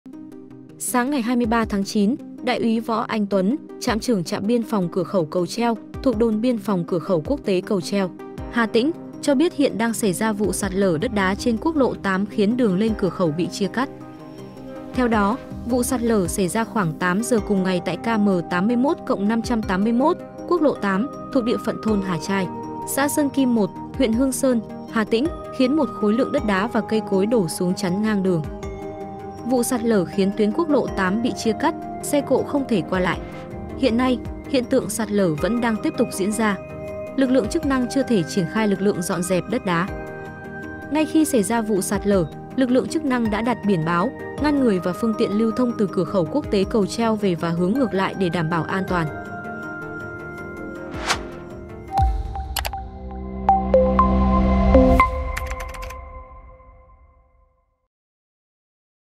Sạt lở chắn đường lên cửa khẩu quốc tế Cầu Treo
Một khối lượng đất đá và cây cối trên núi đổ xuống khiến đường lên cửa khẩu quốc tế Cầu Treo bị chia cắt, xe cộ không thể qua lại.